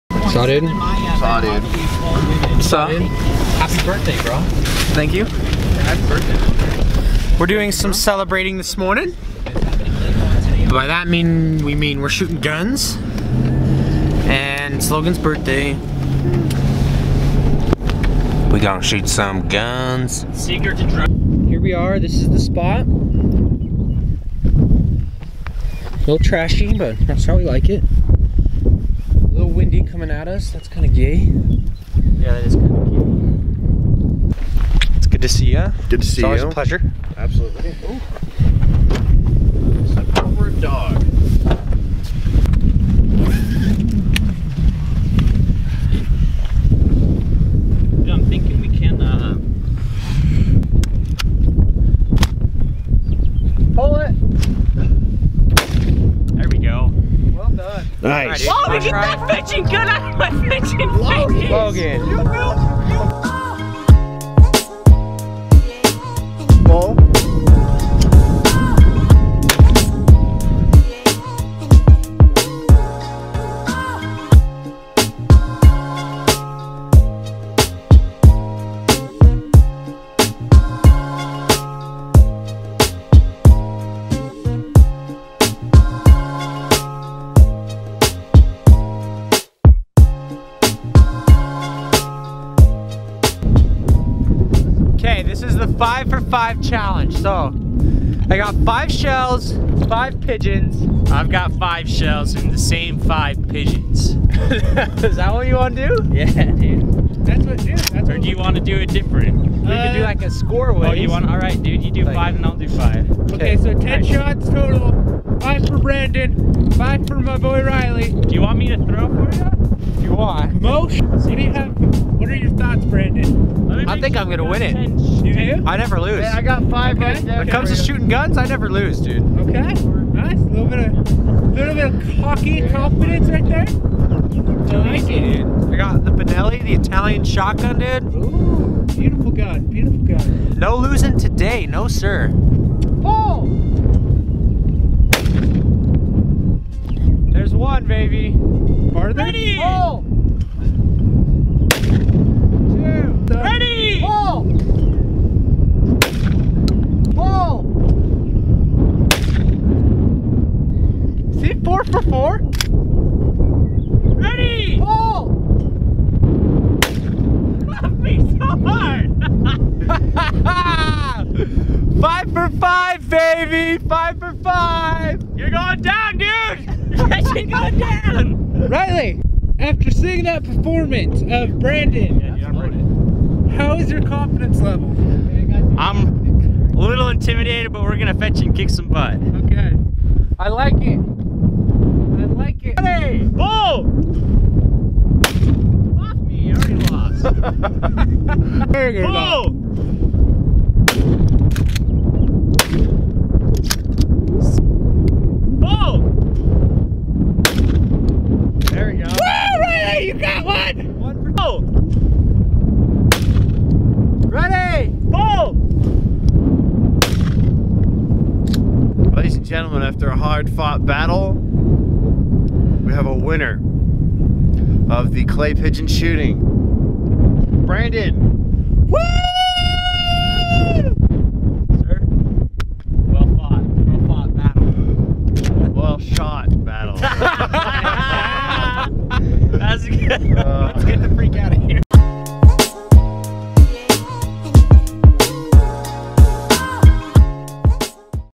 How to properly shoot a shotgun.
Check out a nice friendly competition between myself and Mr. Riley N. HIll as we go head to head in a clay pigeon shooting contest. also..HAPPY BIRTHDAY LOGAN!